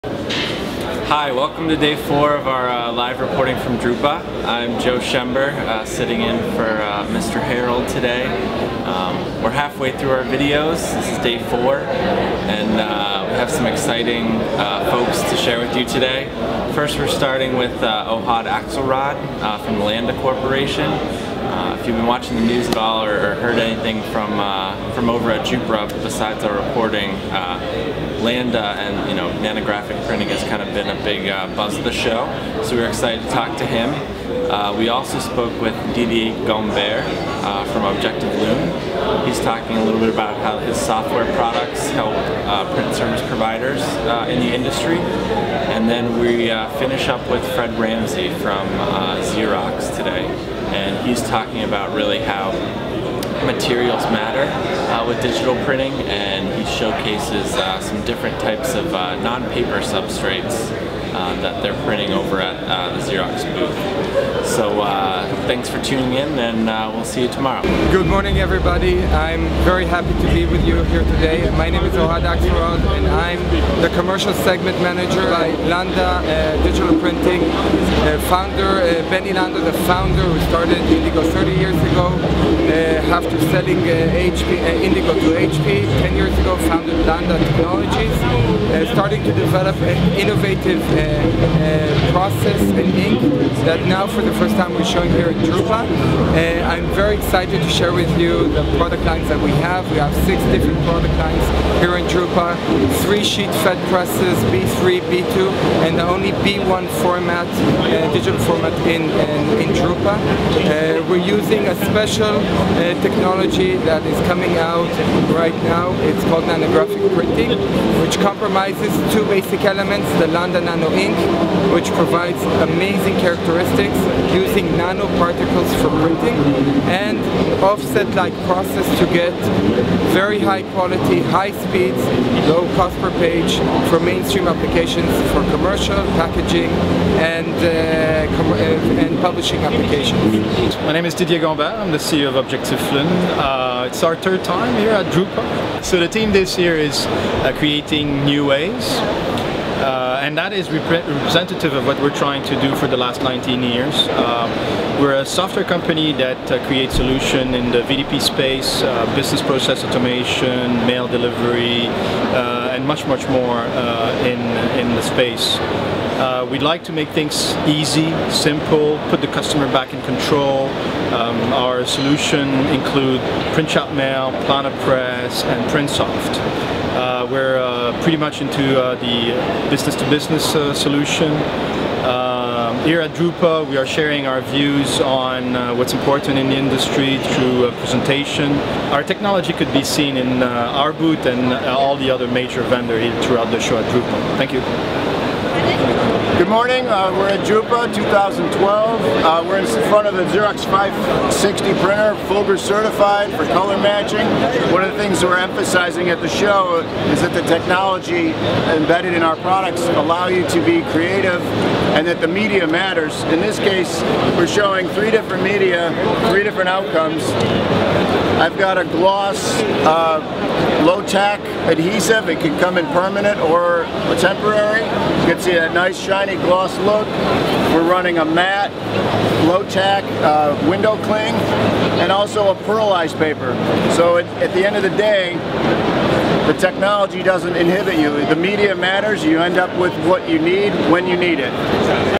Day 4 Drupa 2012
We're over halfway through our 7 days of Drupa coverage, but we're not close to slowing down! One of the biggest attention-getters of the show so far has been Benny Landa and his new nanographic digital presses. We spoke with Ohad Alexrod from Landa Corporation regarding the new technology and process. Then Didier Gombert, CEO of Objectif Lune, spends a few minutes discussing how their B2B VDP and process automation software is helping digital printers simplify their businesses. We conclude today with Fred Ramsey from Xerox talking about how specialty substrates bring higher value applications within reach for a digital printer.